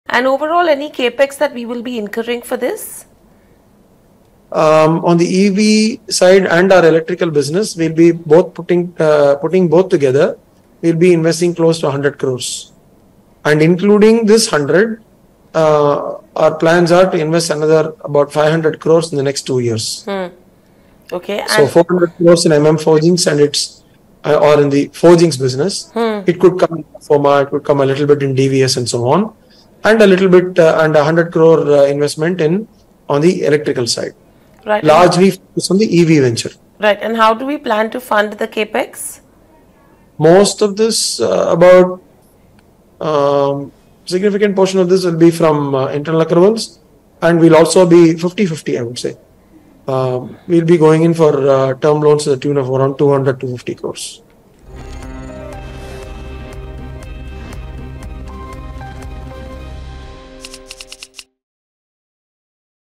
MM Forgings' Vidyashankar Krishnan Outlines The Capex Details And Funding Required | BQ Prime
MMForgings Vice Chairman & MD Vidyashankar Krishnan outlines the capex details and funding required.